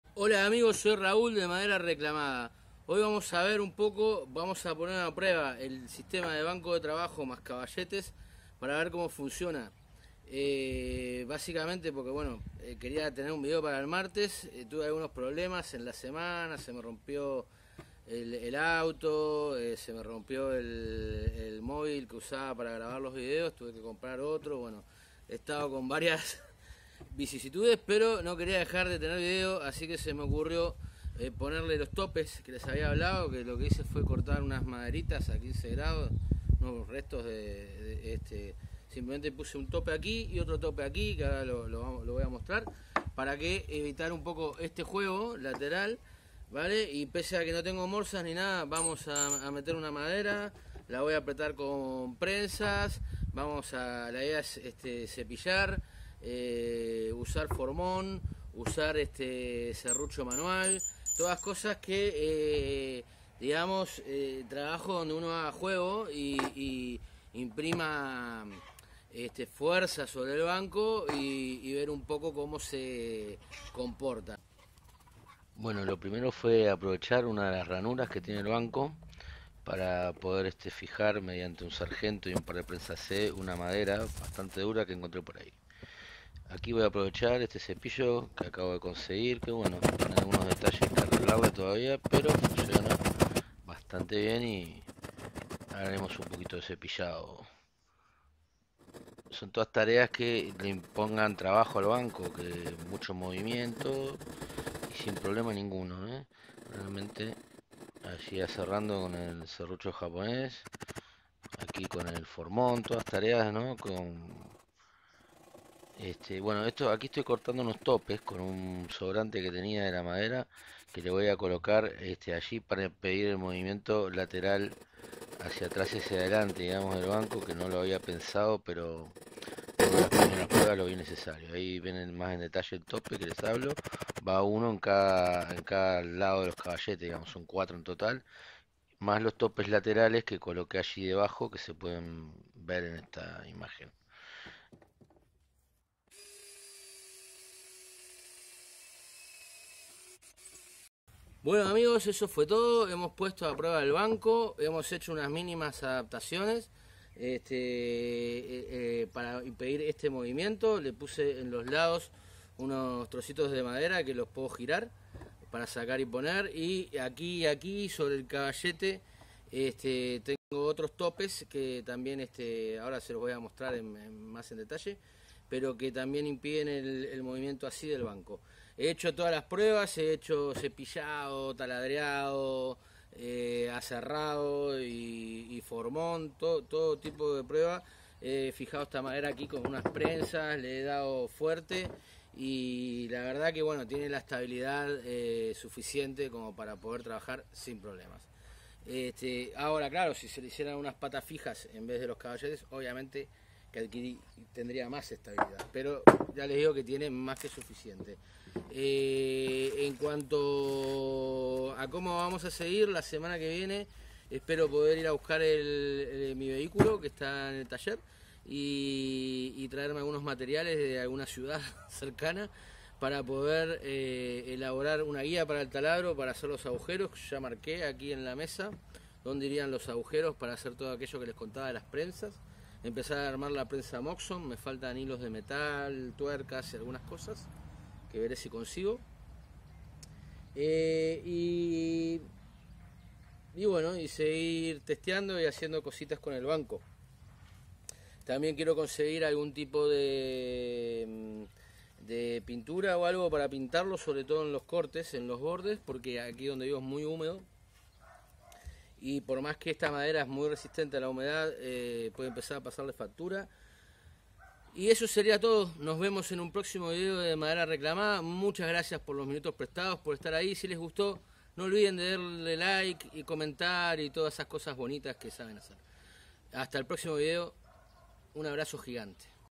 Banco de trabajo, pruebas y topes
Hola amigos hoy realizo unos test de uso al banco de trabajo sobre caballetes y agrego unos topes al diseño para darle más estabilidad aunque sin estos igual el resultado era bueno.